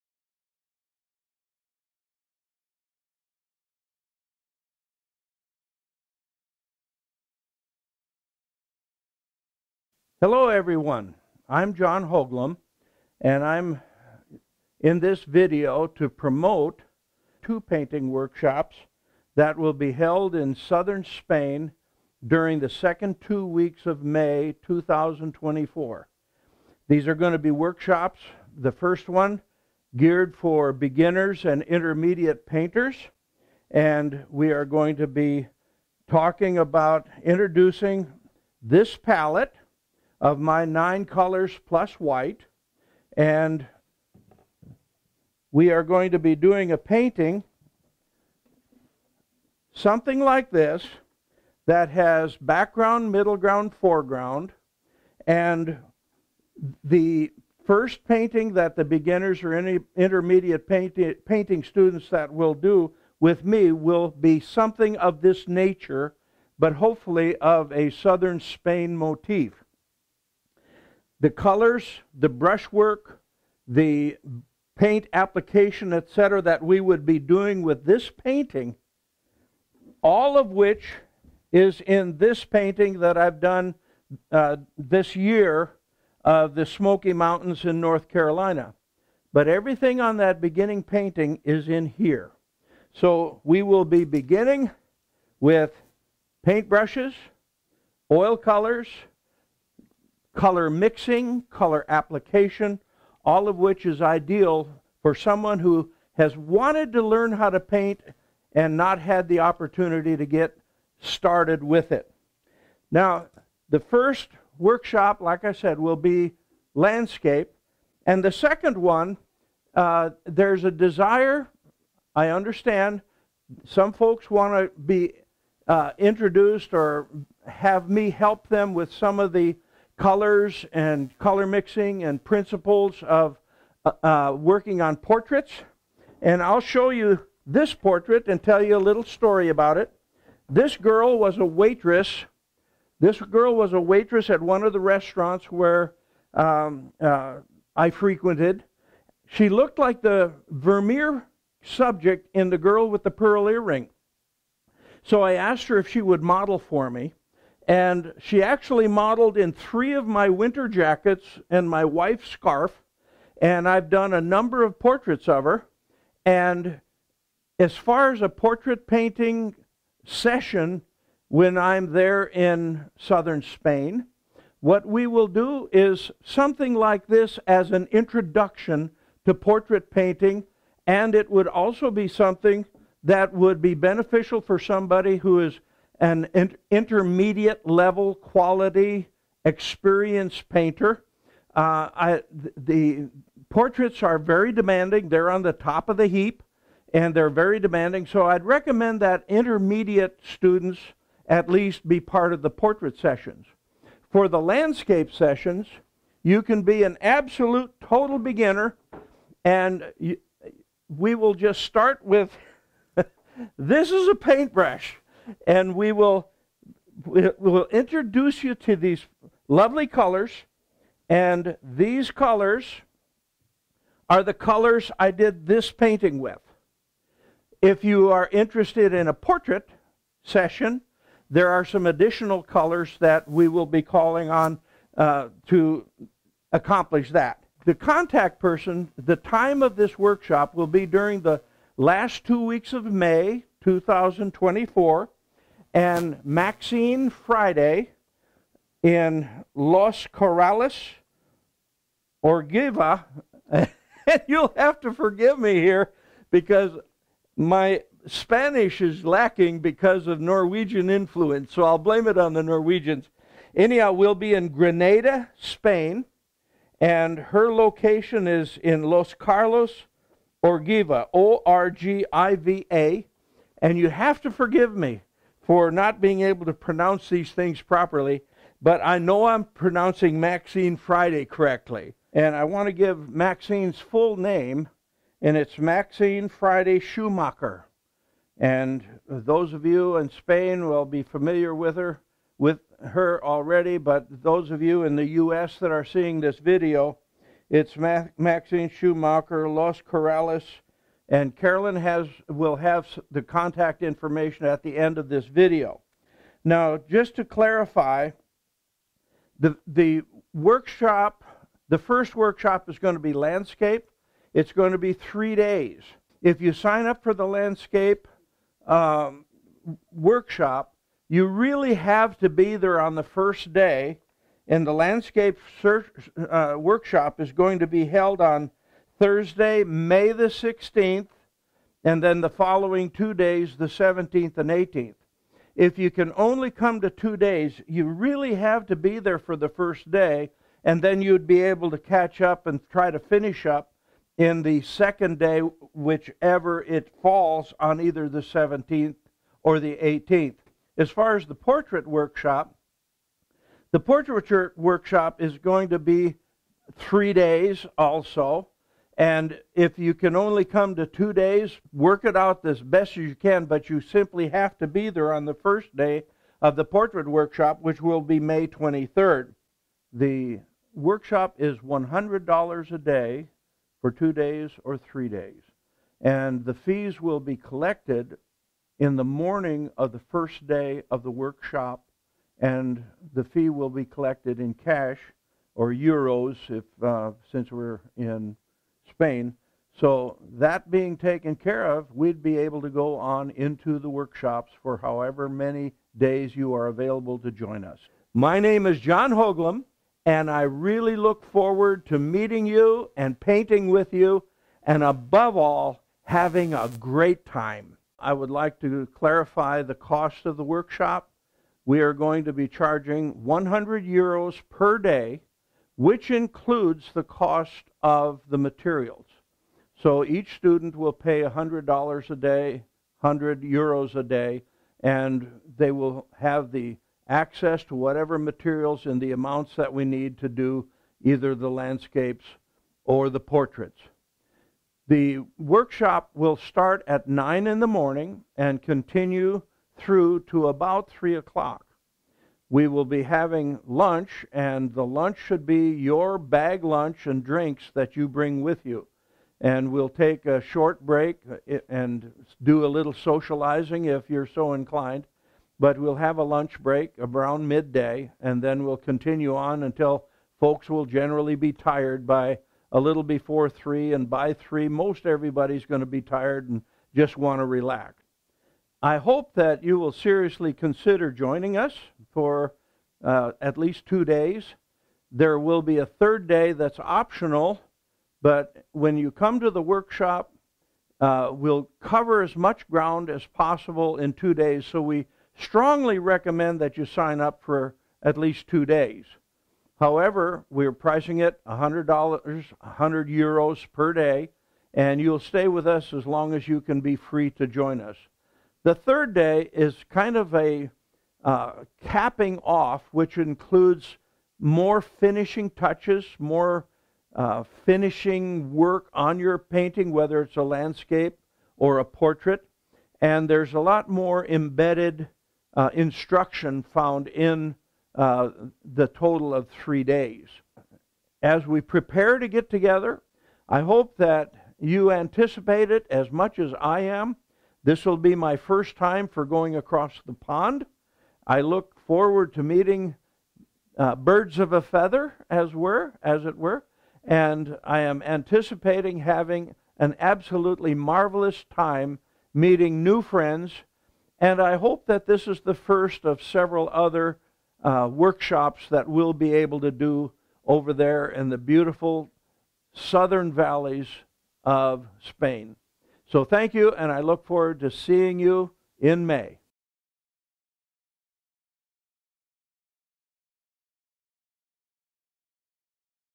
Southern Spain Oil Painting Workshops
Oil Painting Portrait Workshop for Intermediate Students and Southern Spain Landscape Oil Painting Workshop for beginner and intermediate students. Located at Los Corrales, Sortes, Orgiva, Spain, in in Andalucia. Taught by Jon Houglum, professional artist from North Carolina, USA, a teacher of oil painting for over 40 years.